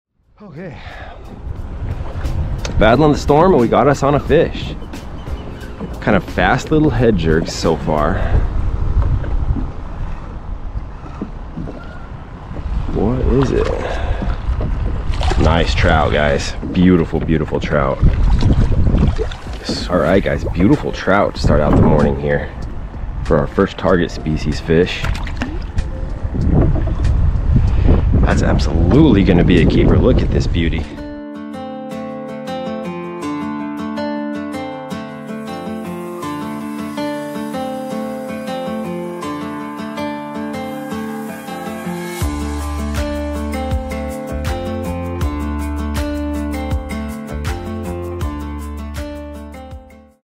💣🌮 Turning a SPECKLED TROUT into BOMB TACOS!
I caught a perfect speckled trout to eat and made some amazing fish tacos. Check the full video CATCH and COOK.